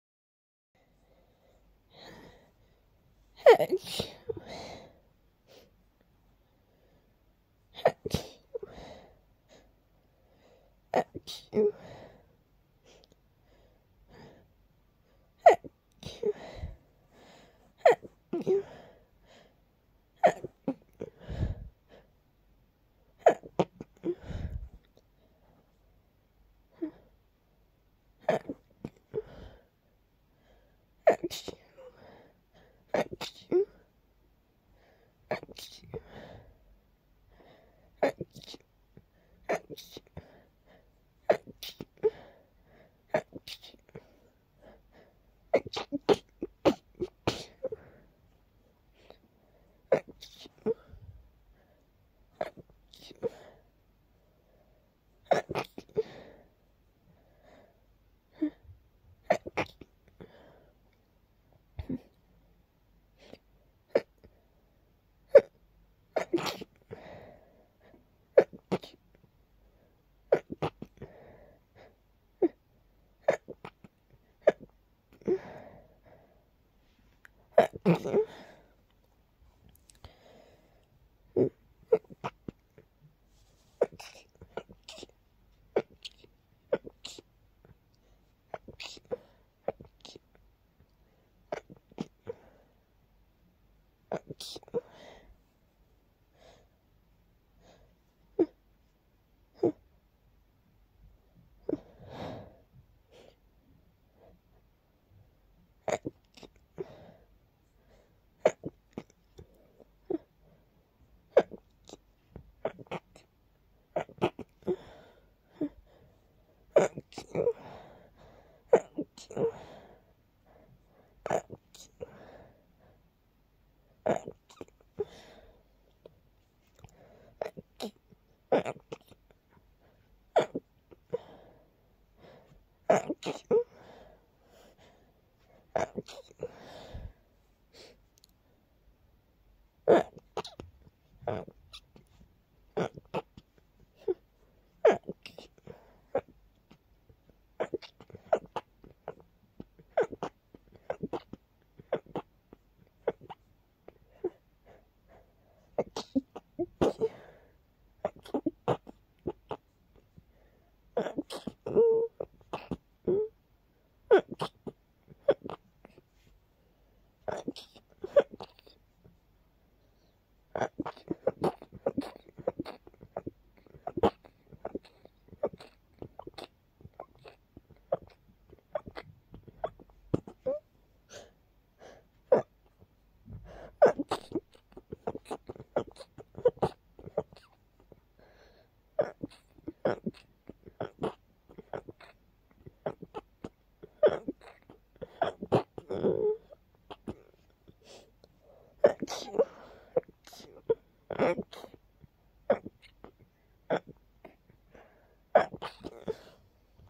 Soft dusty sneezes (and a little rapid fit)
I was cleaning a bit, then I got to a part of the room that hasn't been cleaned in a long time. I had to move some heavy things around to get there, and the amount of dust surrounding it was quite astonishing. I managed to clean it all! but of course for a price...